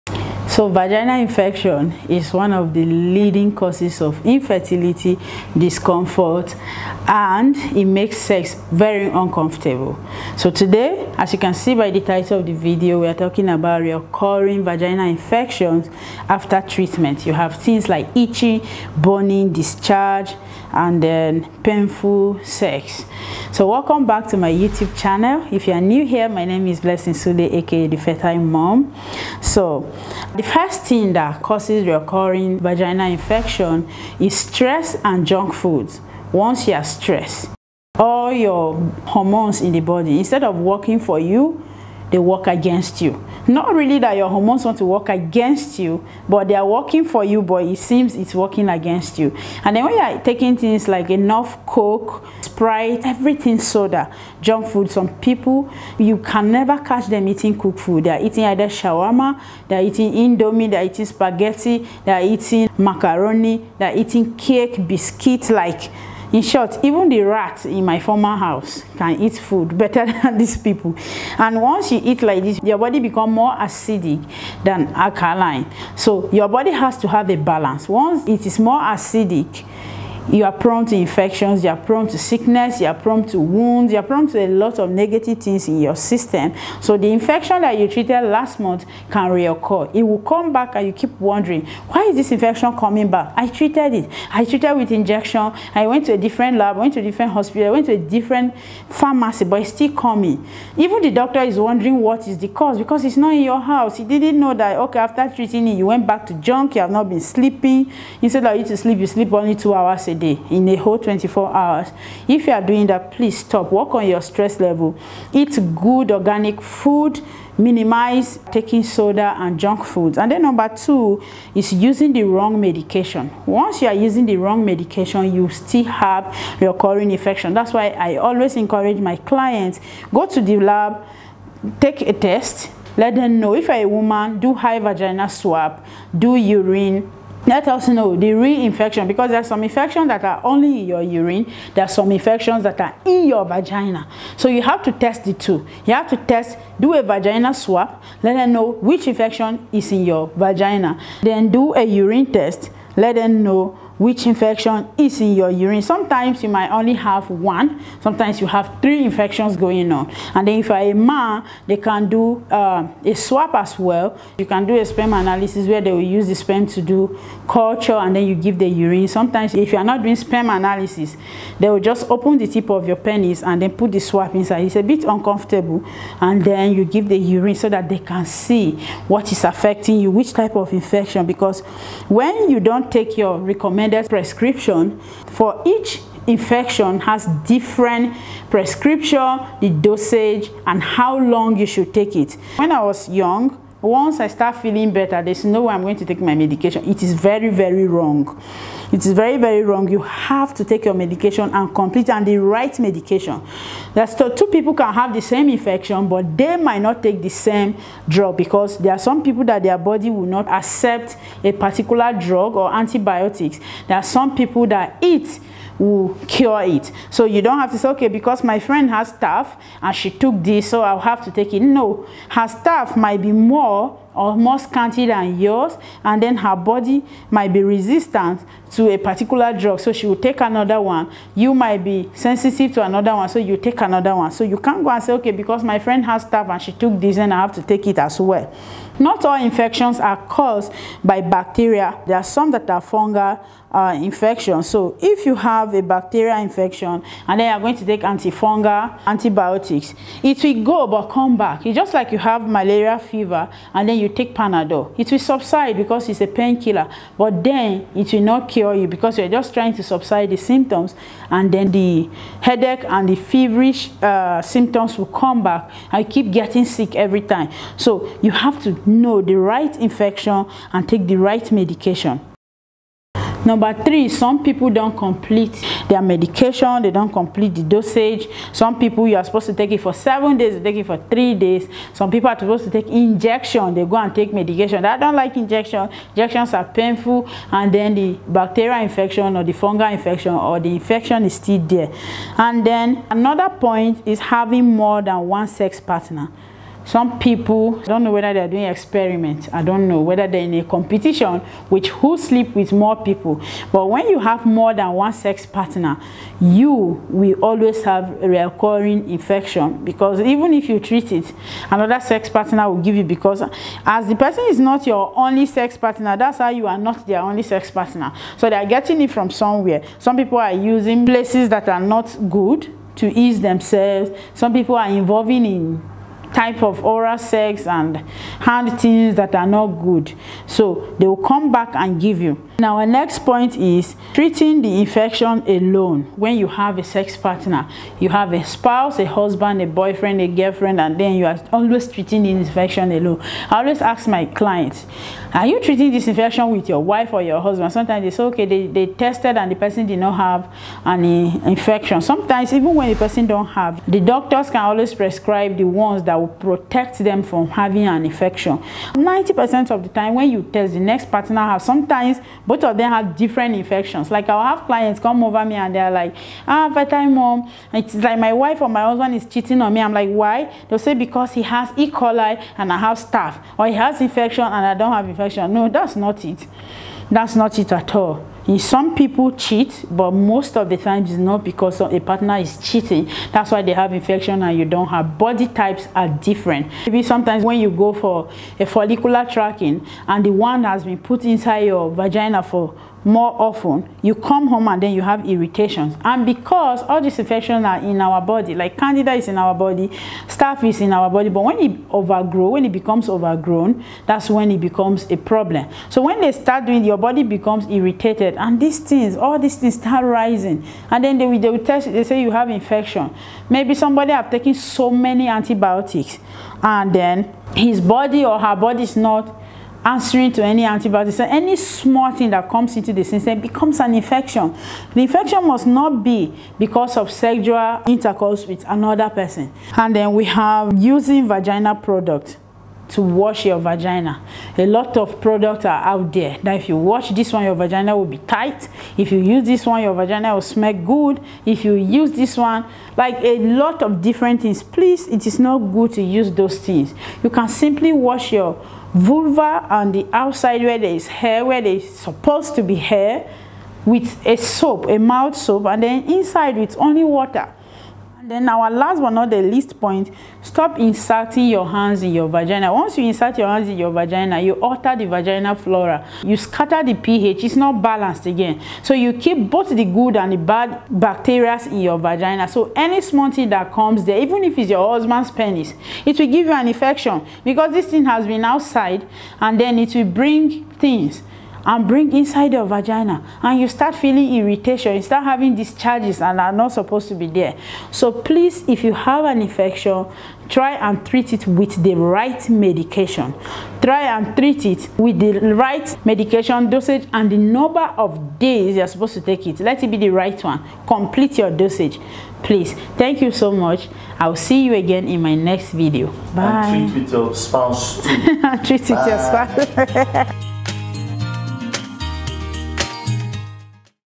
Why do I keep having vagina infection? (Best treatment for vagina infection)
Many people feel recurring vagina infection is only caused by having multiple sex partners (or cheating). There are different reasons why you may be having vagina infections always. In this video, I revealed the different causes of vagina infections, the best ways to treat vagina infections and what to do to prevent vagina infections.

0:00 - 0:30 Intro
0:30 - 7:59 Causes of vagina infection (and ways to treat/prevent vagina infection)
8:00 - 8:34 Conclusion